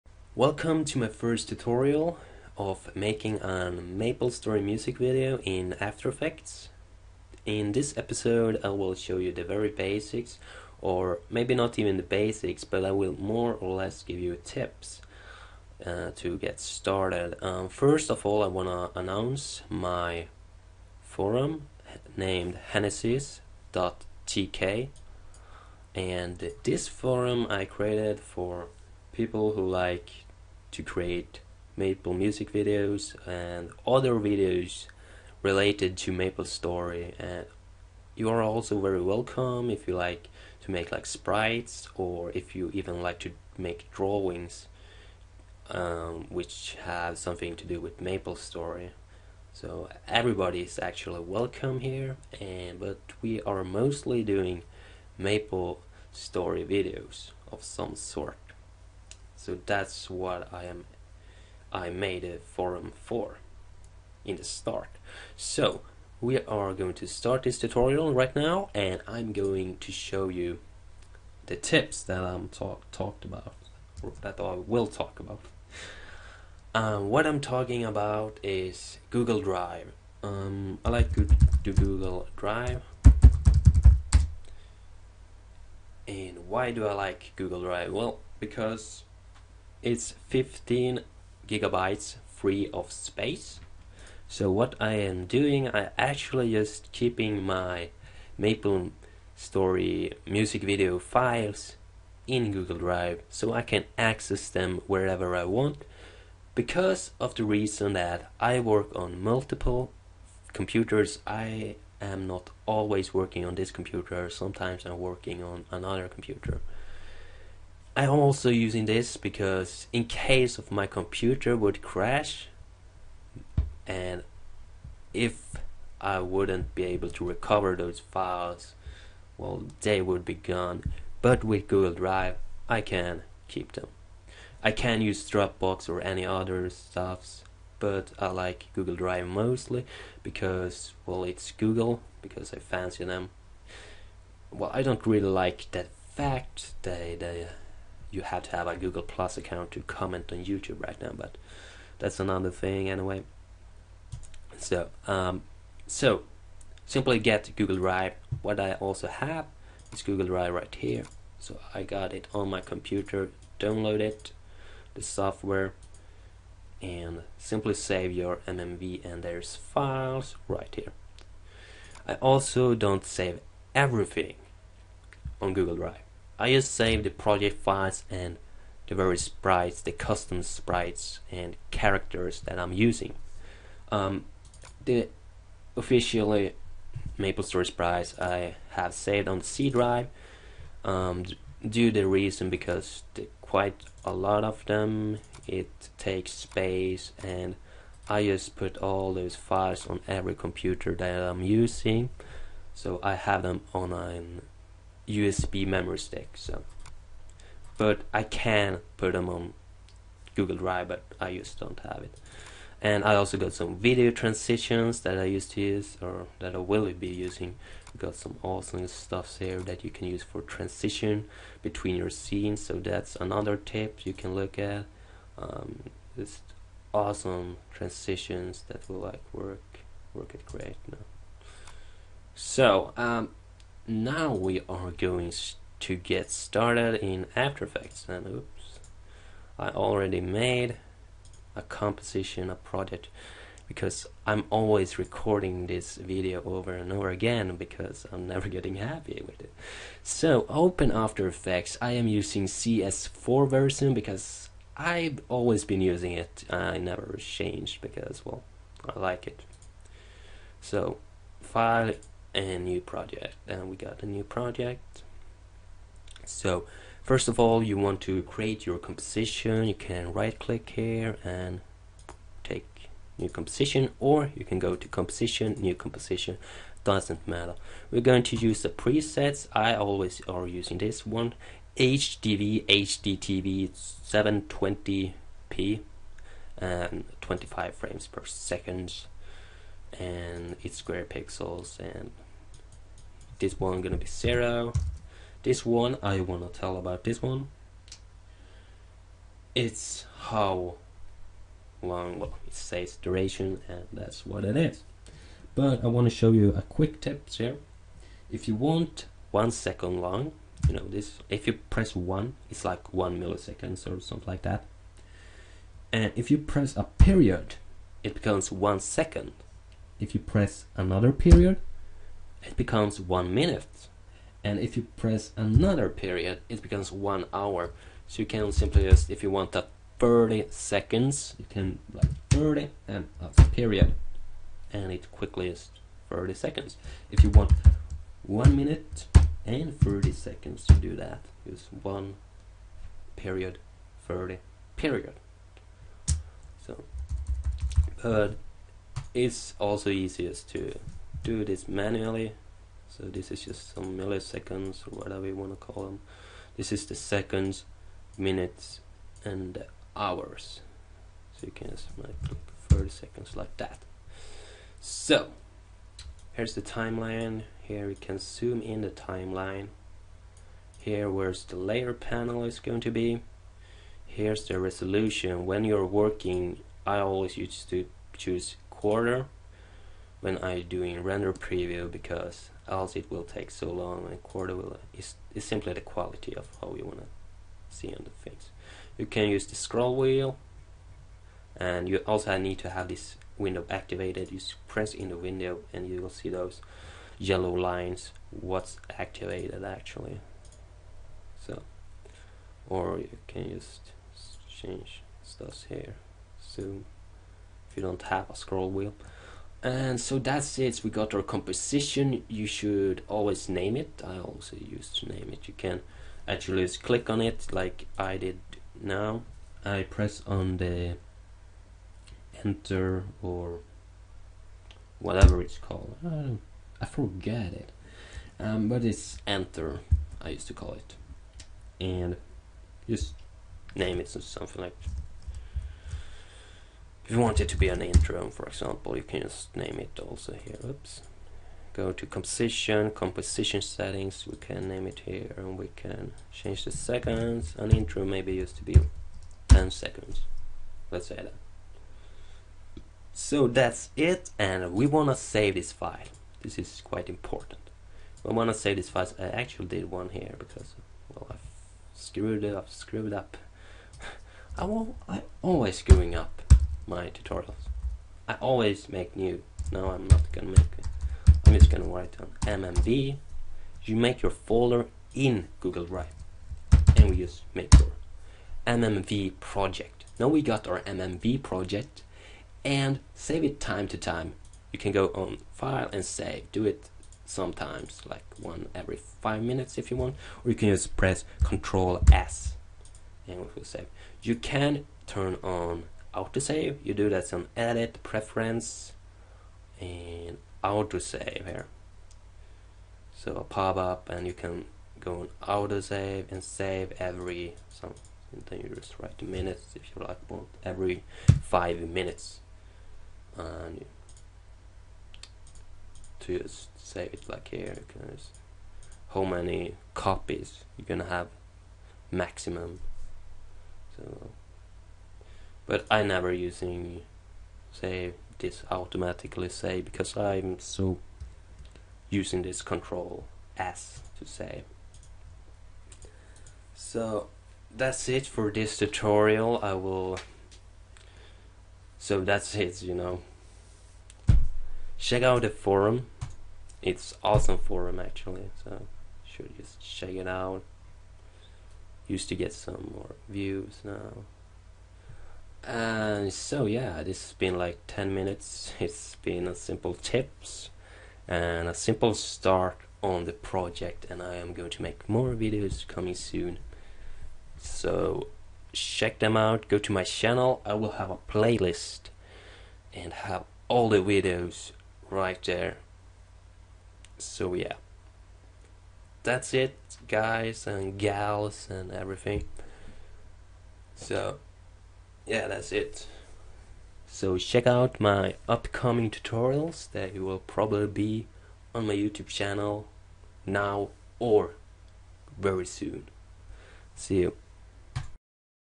[TUTORIAL] MMV in After Effects Part 1 - TIPS and start
Giving you tips to use Google Drive and getting started with the very basics!